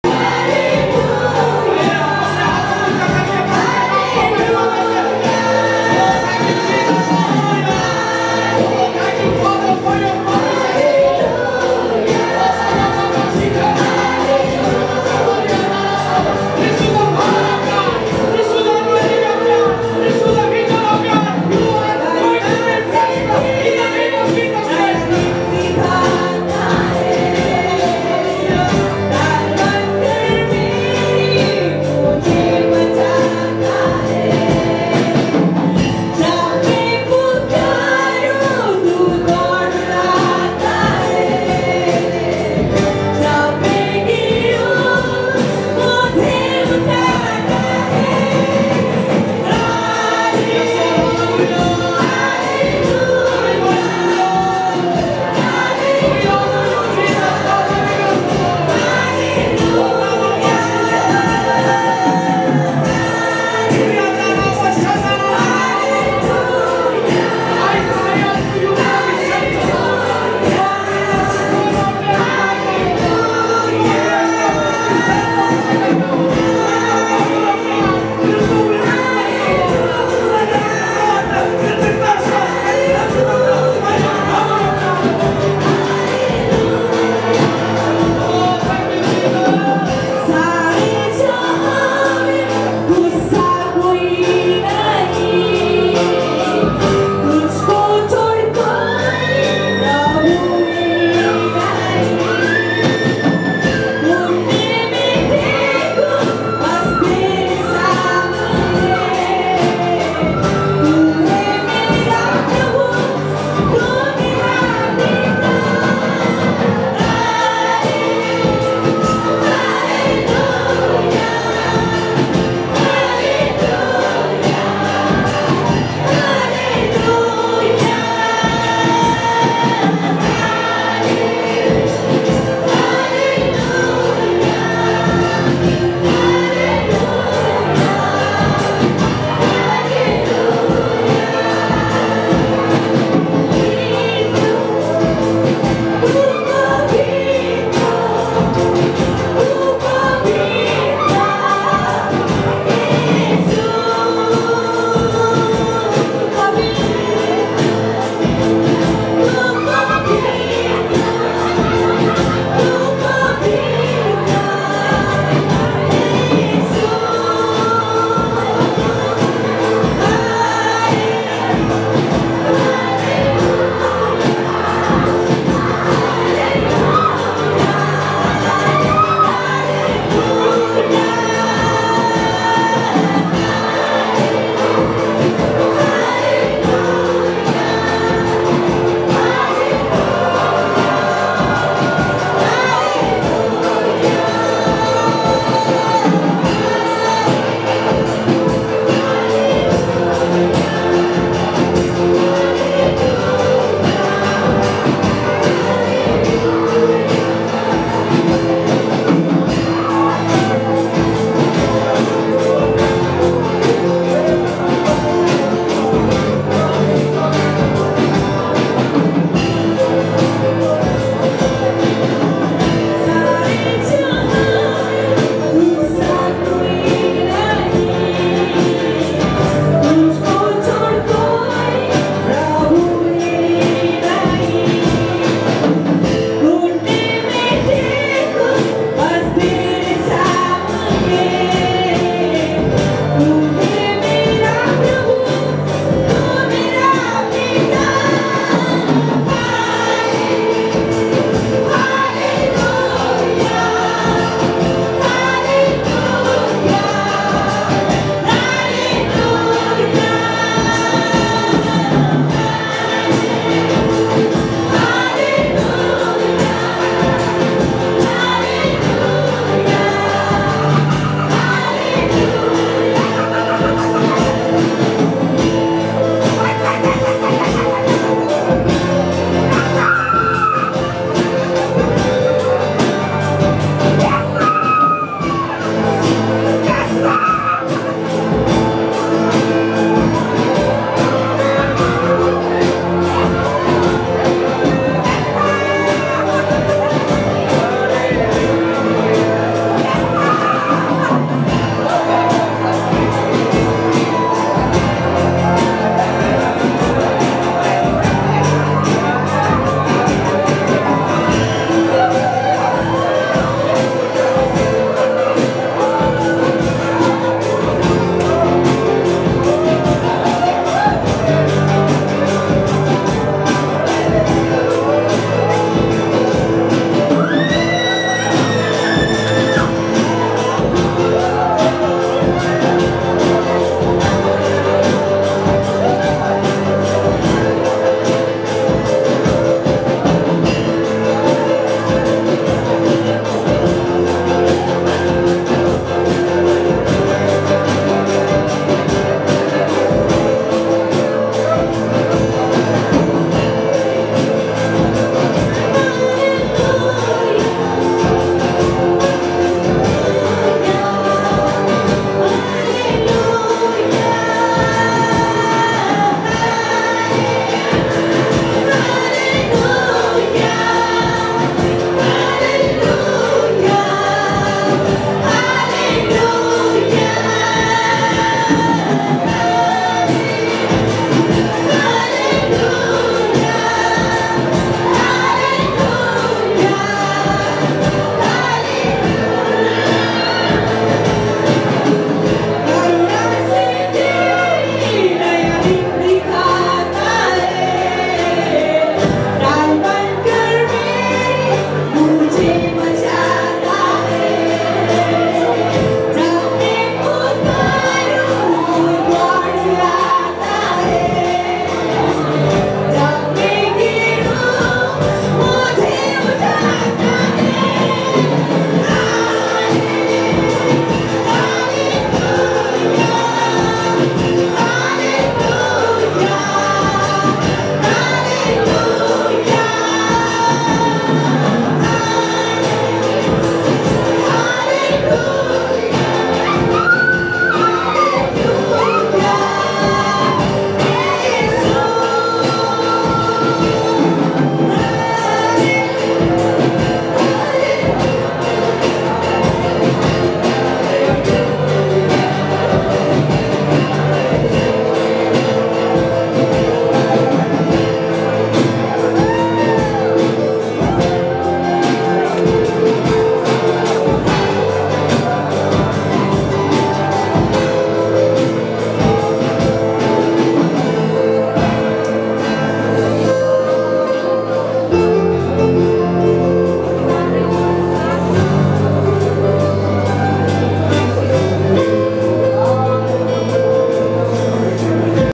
Deep prayers in Revival Conference 2080
Deep prayers in Revival Conference 2080 in Ashish Church, Pahalmanpur, Kailali.